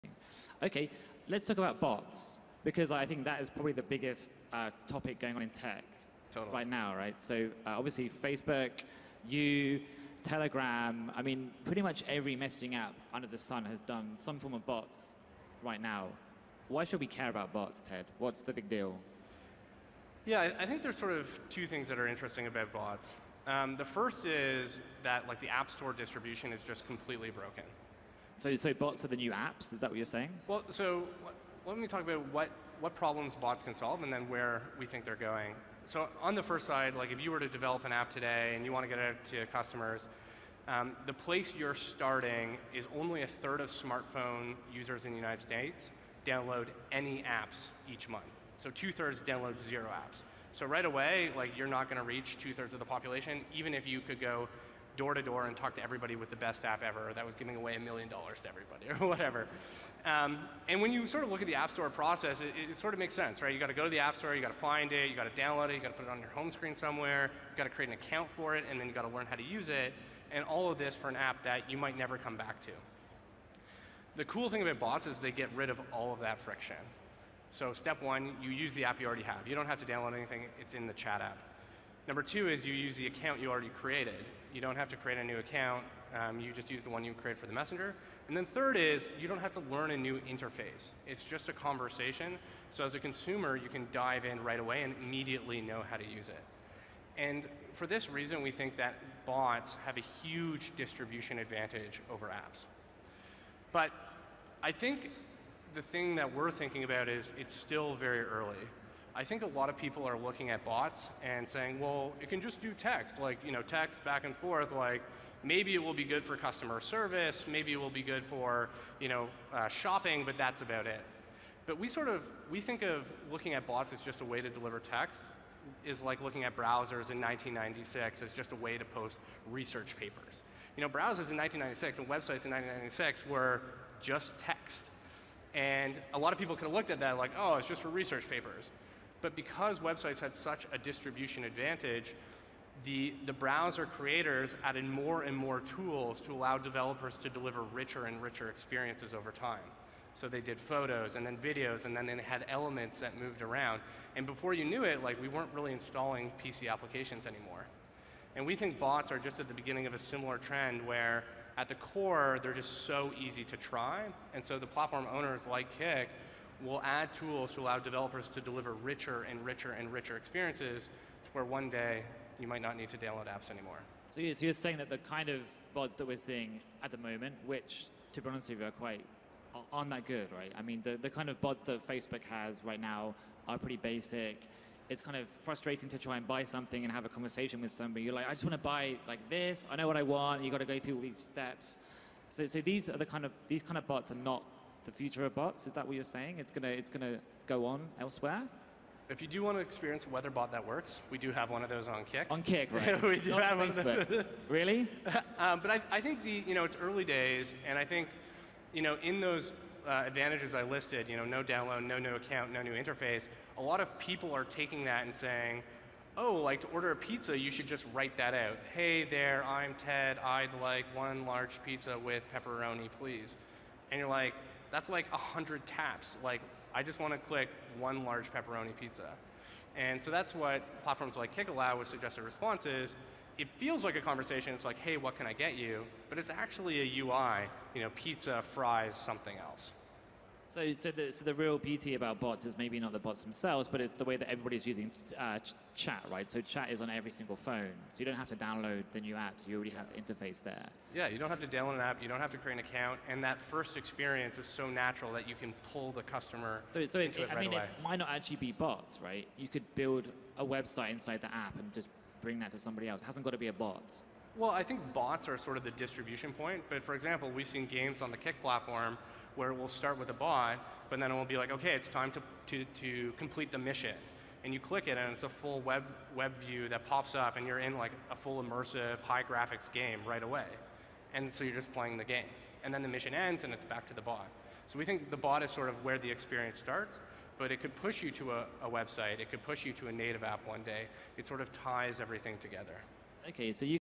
Kik CEO discusses bots at Disrupt NY
Inside the Chat Wars with Ted Livingston (Kik)